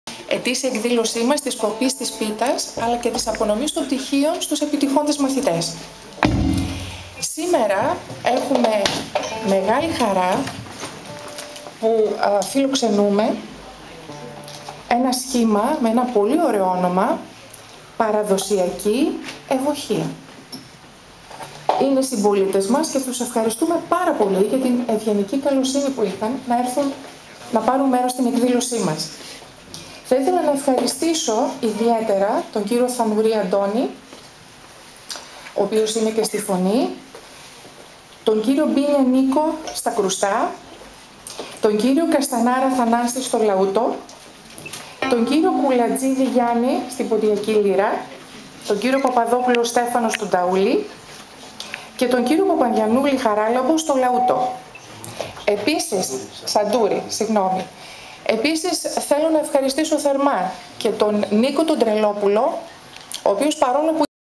ΑΠΟΝΟΜΗ PALSO EDEXCEL 2013 ΠΙΕΡΙΑΣ
Η απονομή ξεκίνησε με μουσική και τραγούδια